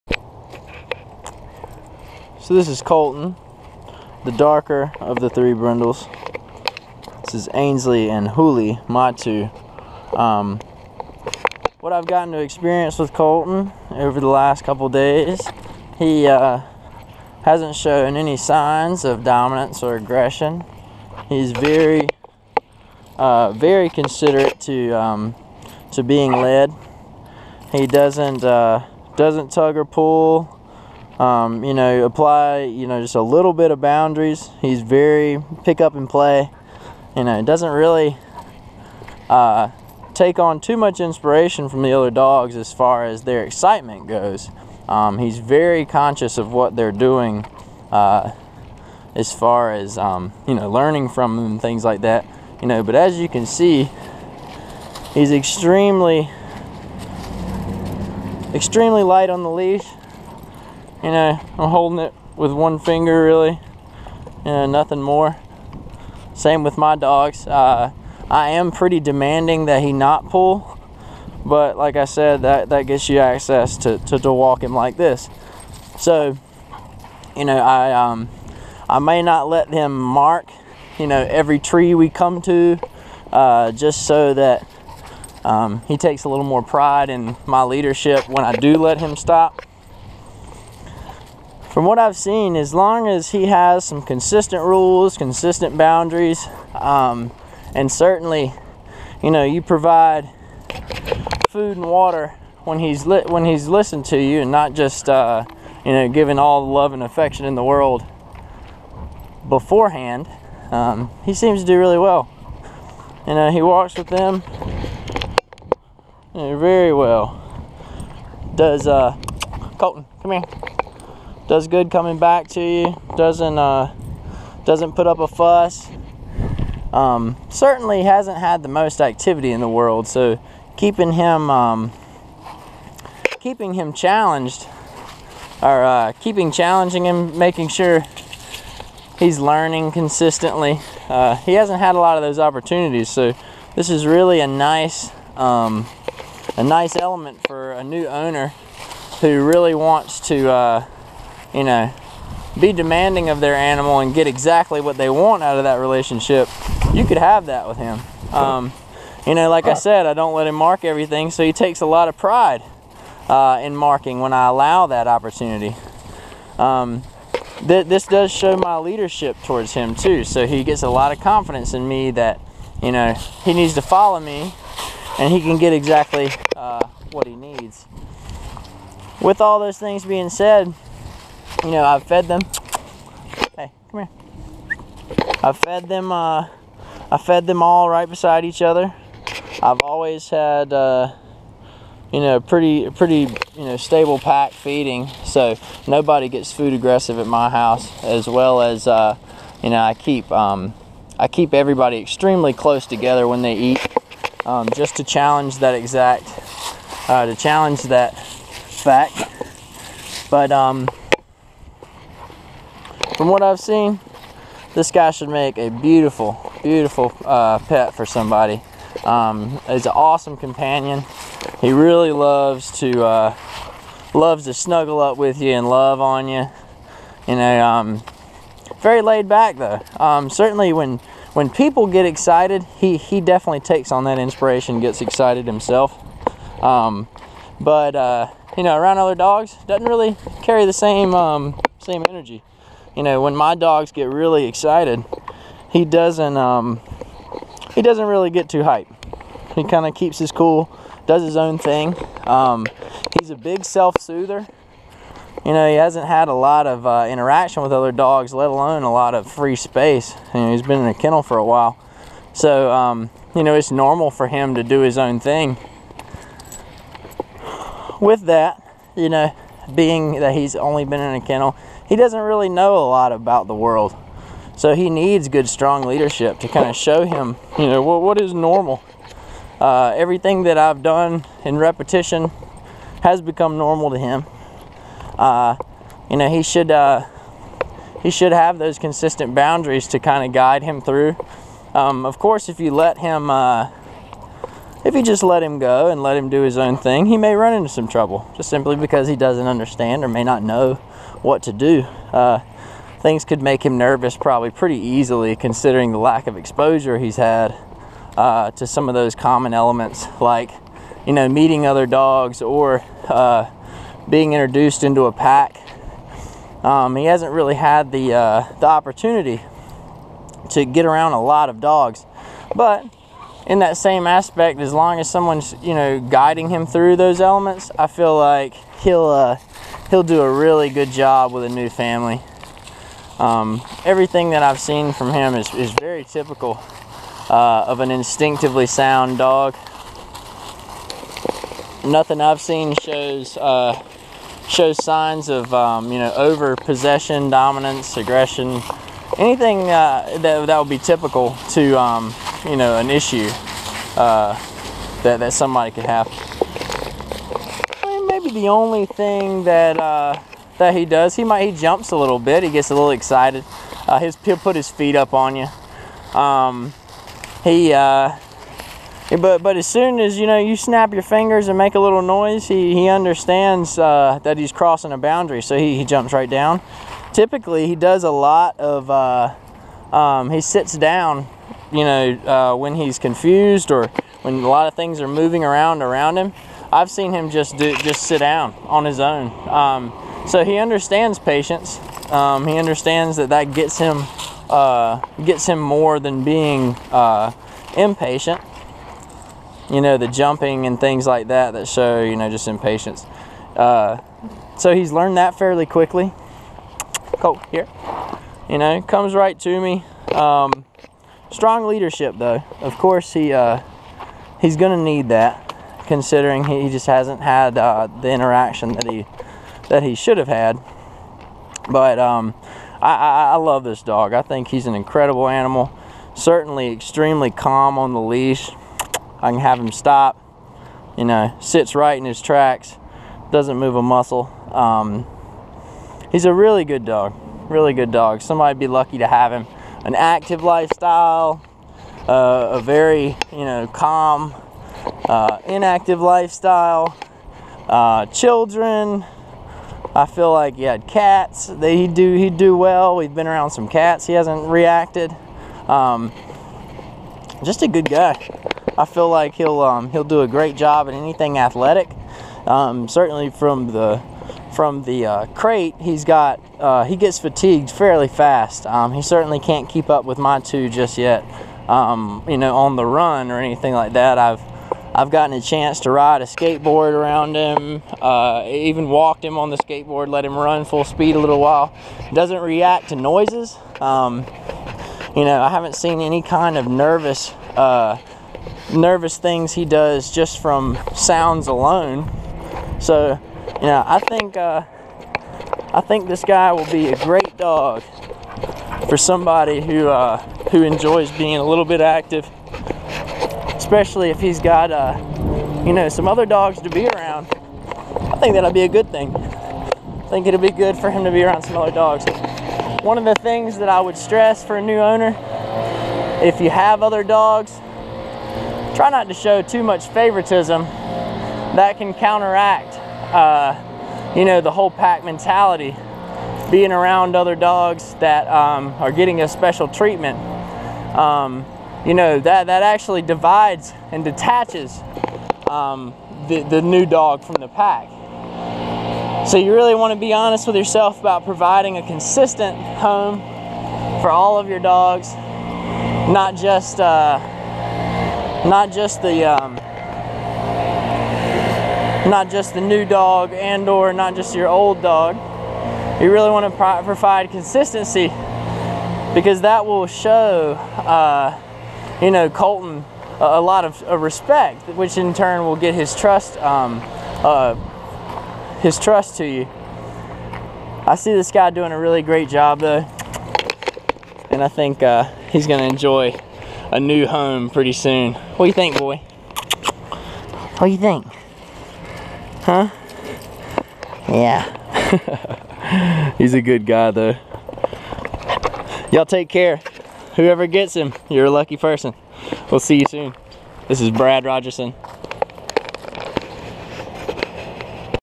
Colton Evaluation
Colton
Shepherd Mix

Up for Adoption!
Looking for a good home!

Colton has been living in a Crate for the last 8 months.
His life has revolved around a small space for a good portion of his life.
One of my friends was nice enough to save him from a kill-shelter on his last week before being put down. Now he has a great chance to find peace.
I took some time to evaluate his behavior and nature.

He is wonderful. I feel he will make someone very happy one day.